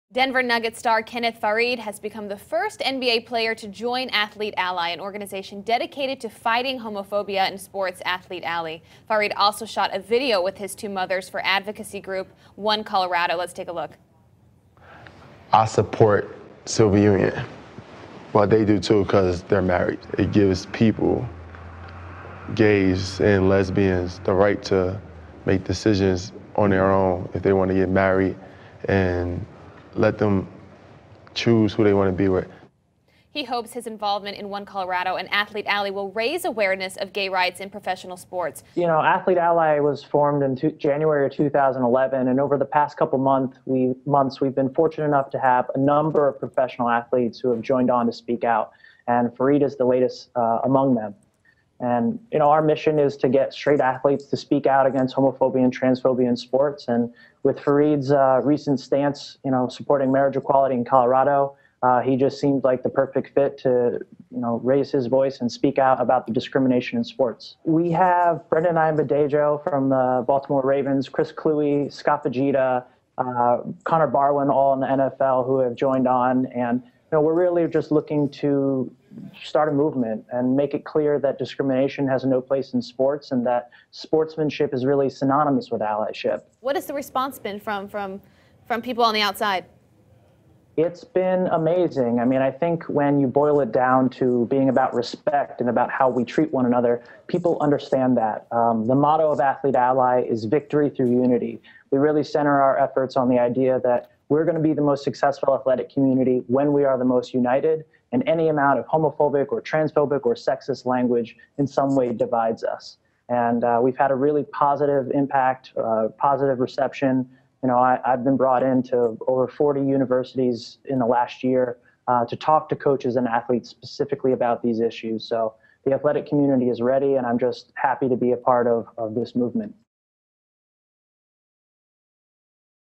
NBA Star Kenneth Faried Joins Gay Rights Group
NBA star Kenneth Faried is an advocate for same-sex marriage and a fierce supporter of his two moms. He is the 1st NBA player to join the gay rights group Athlete Ally. Found of Athlete Ally Hudson Taylor joins Abby to talk about the organization.